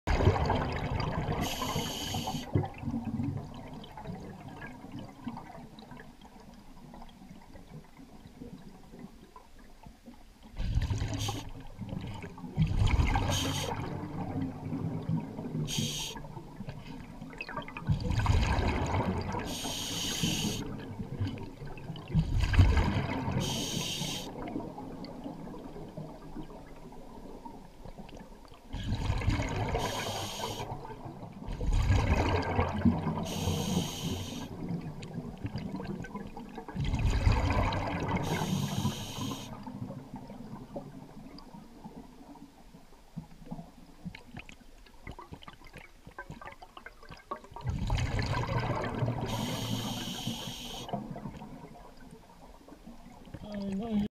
Common Lobster - Scotland
The common lobster is a species of clawed lobster which is found all around the British Isles, as well as throughout the rest of Europe. It is an extremely commercially valuable species which plays an important role in many fisheries around the UK. Scotland.